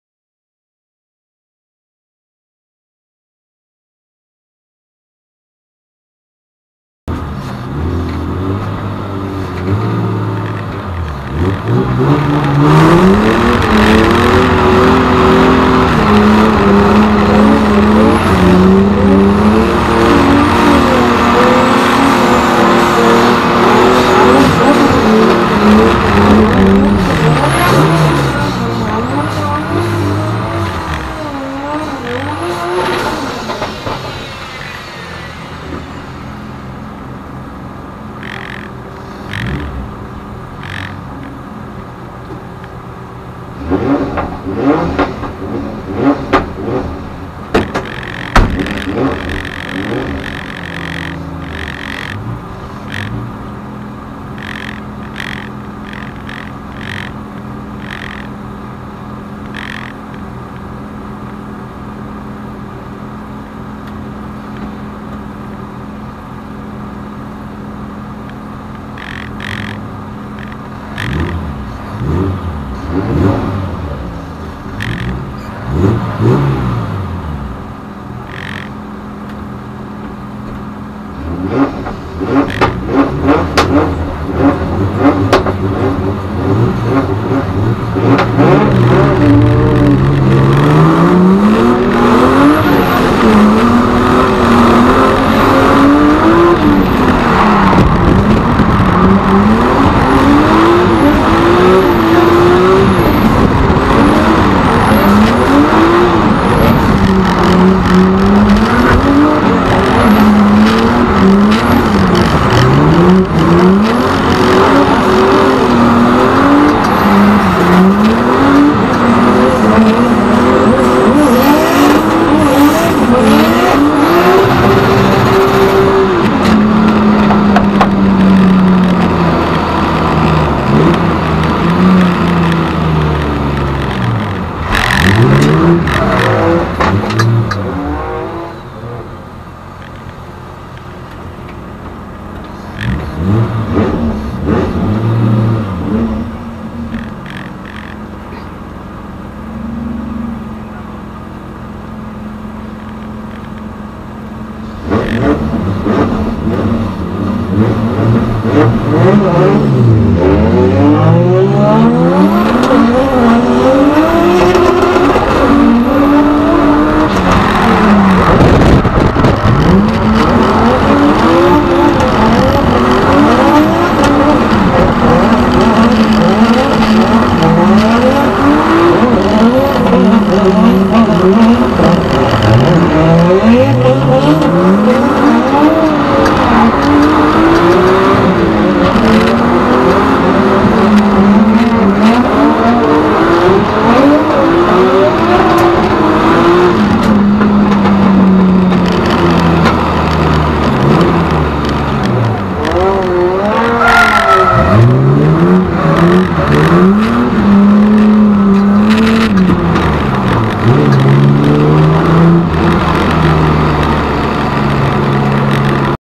Lwi Edwards : BDC2019 Rnd2 : Driftland : Top 8 Battle : Lwi Vs Cowley
Lwi Edwards - UK Pro Am Drifter / Irish Drift Games drifter
British Drift Championships Round 2 Driftland, Scotland
Top 8 Battle with Martin Cowley
Martin Cowley wins by small margin